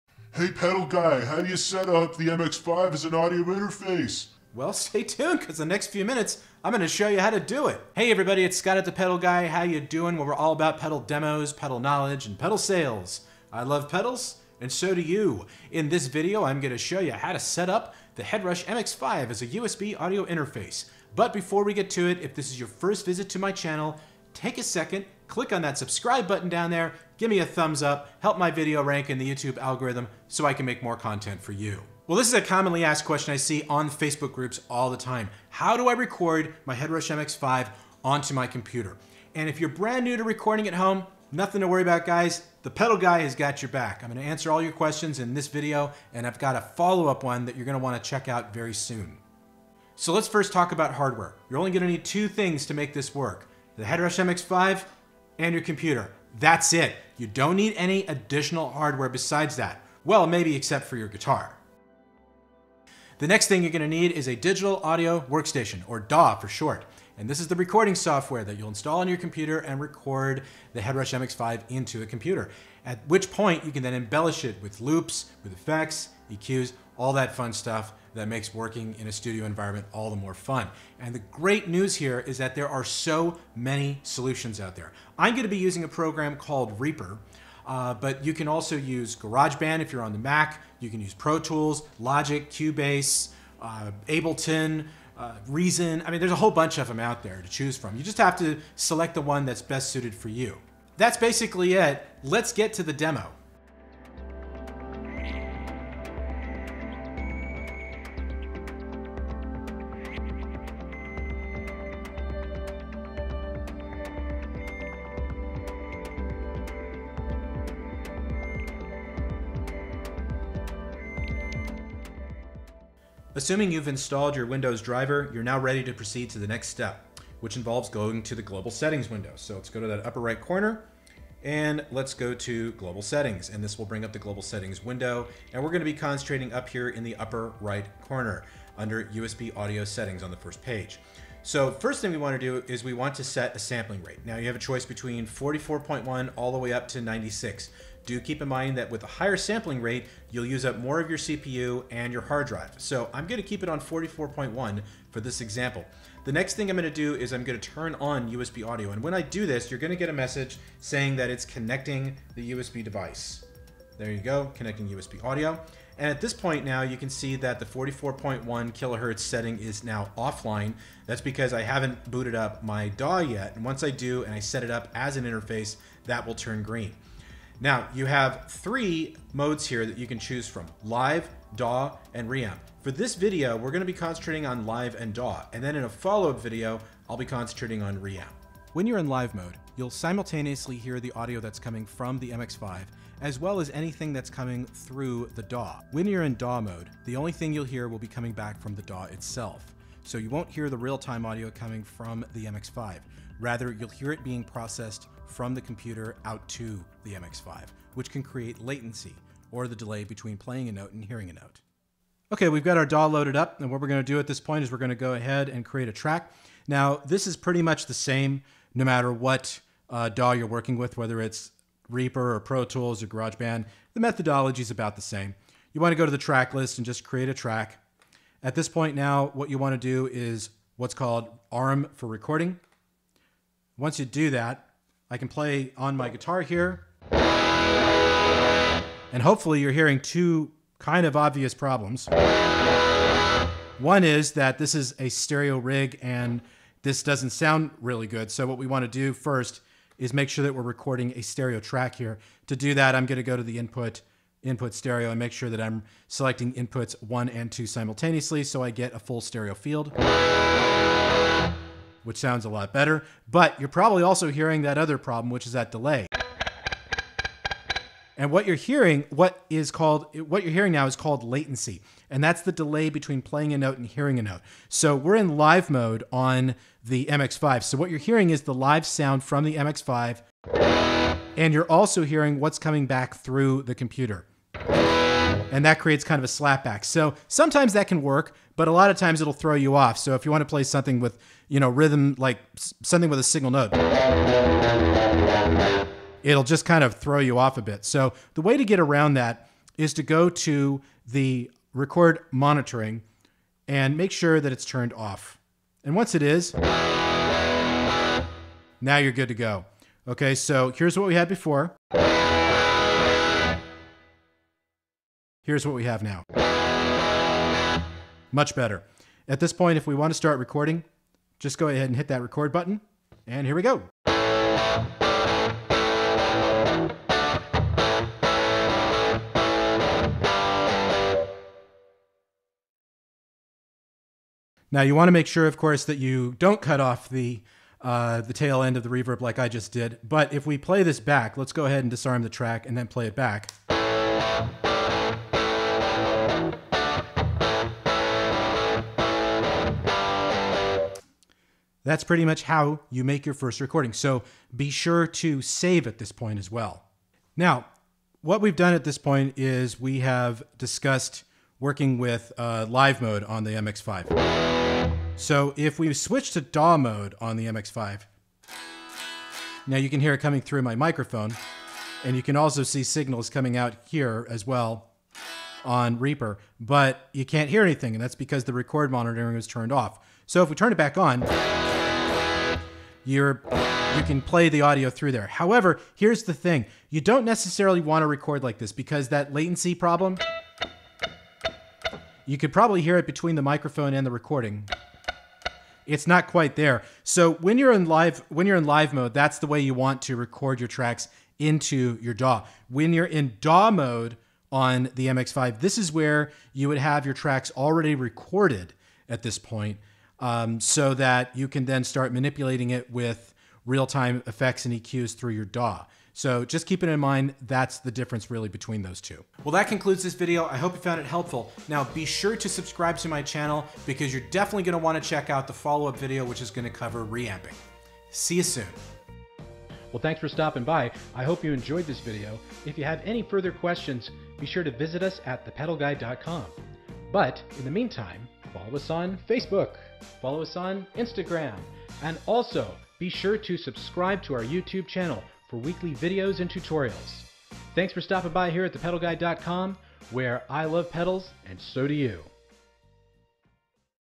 Start Recording with the Headrush MX5
Welcome to the first of a two part series that covers how to start recording the MX5 into your computer. There's a lot of confusion around this topic so this seemed like a good time to cover it. The MX5 is a fantastic USB Audio interface for your favorite recording software, also known as a DAW (Digital Audio Workstation). You really only need the MX5, your computer, and one of several different DAW solutions out there like GarageBand, ProTools, Logic, Cubase, etc. In this video, I'm using Reaper, which is a great DAW for the money.

If you're using Windows, be sure to download and install the MX5 Windows Audio Driver from the Headrush

0:00 Intro
0:49 Hardware and Software
2:02 Setting up USB Audio
3:31 Let's Record